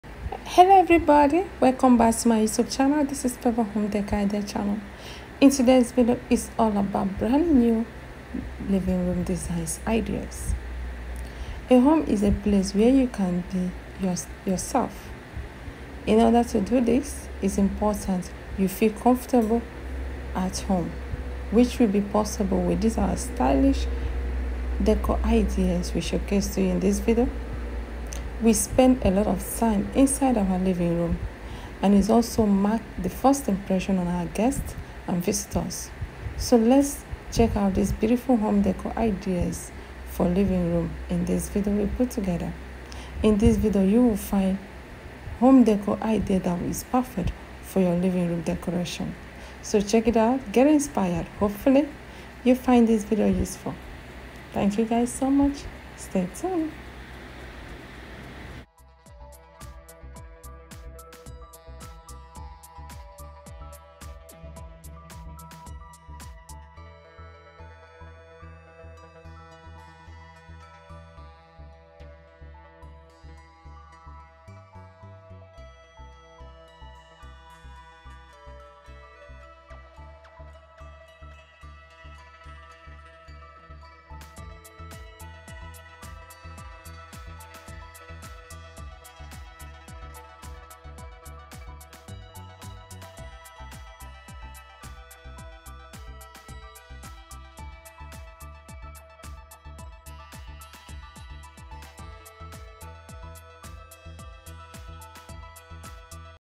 Newest Ways To Designs Your Living Room Ideas
Newest way to design your living space.
@Favour Trendy Things Tv

A home is a place where we can be our true selves. In order to do this, it is important that we feel comfortable at our home which will be possible with our stylish décor Ideas. Showcase to you.

We spend the lots of time inside our living rooms and it also marks the first impression on guests and visitors. So, let’s check out beautiful home décor ideas for our living rooms.

In this video we will find home décor ideas that will be perfect for your living room decoration.

Fellow us on Twitter: @favourideas

Voice over use is my voice: Favour Home Decor